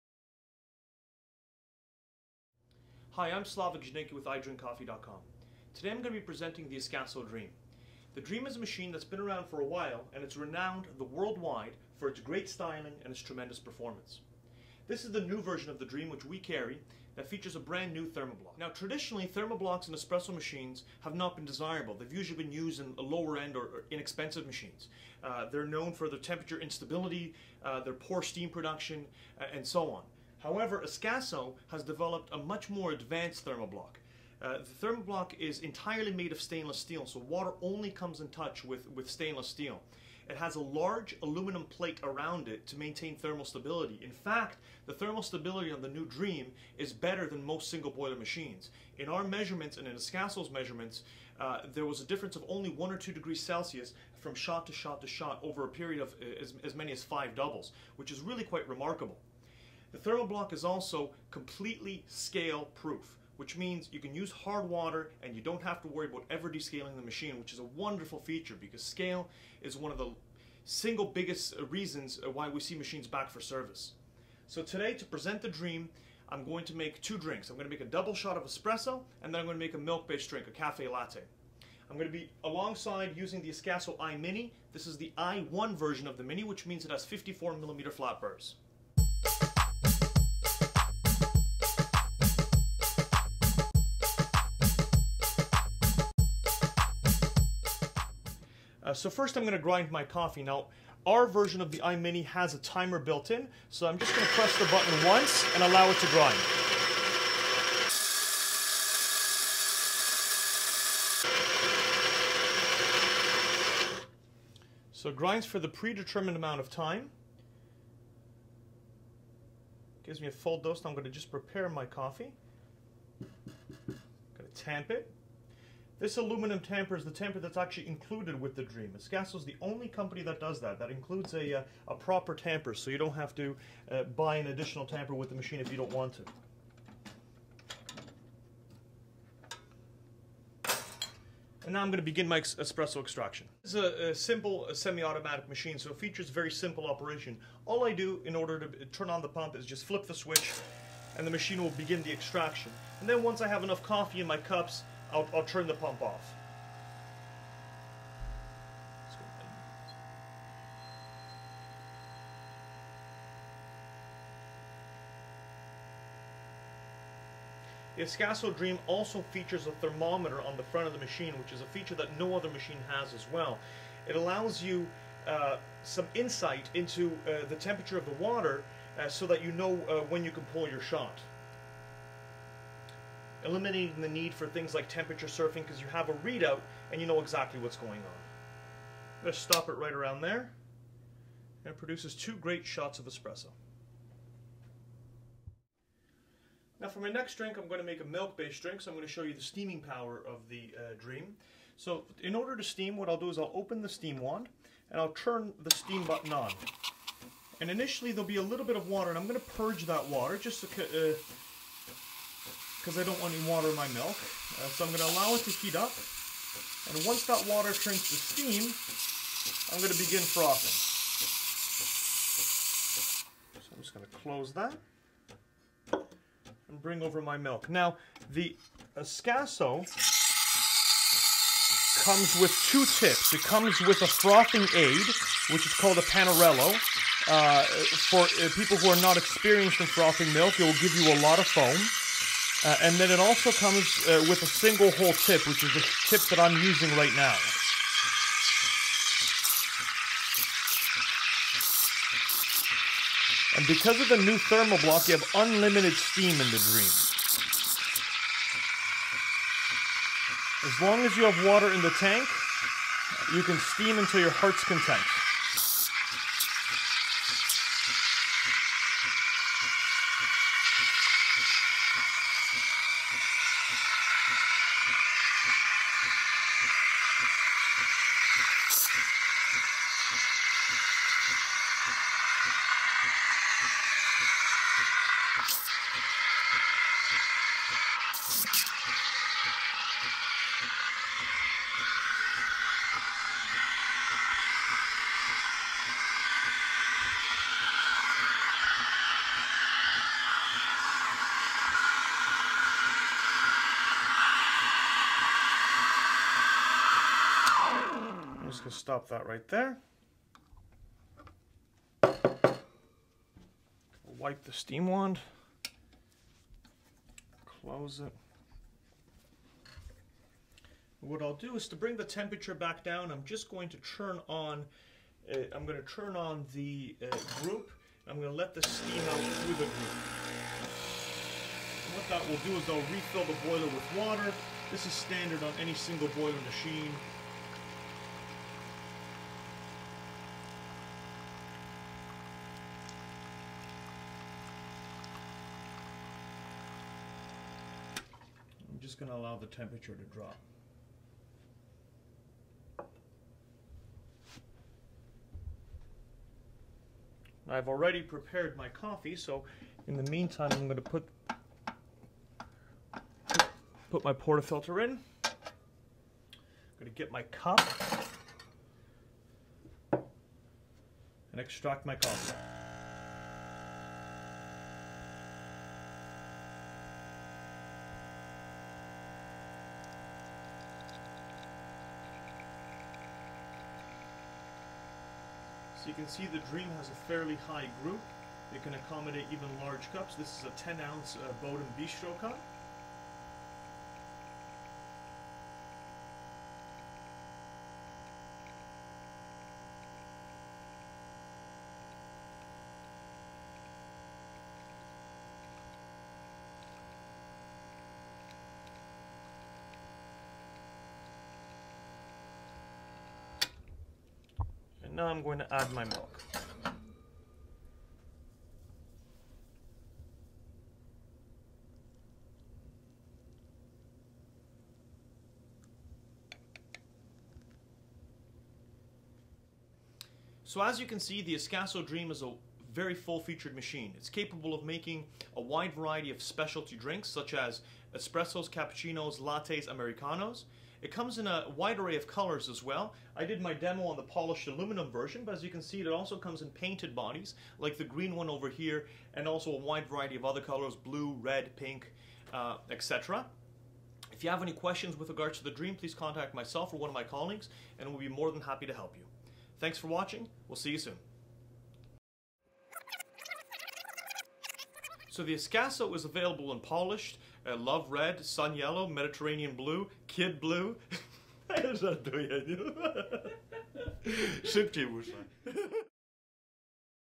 Ascaso Dream UP Preview and Demonstration
In this video Slawek demonstrates the new version of the Ascaso Dream espresso machine.  He makes two sets of drinks, first a double espresso and second a latte.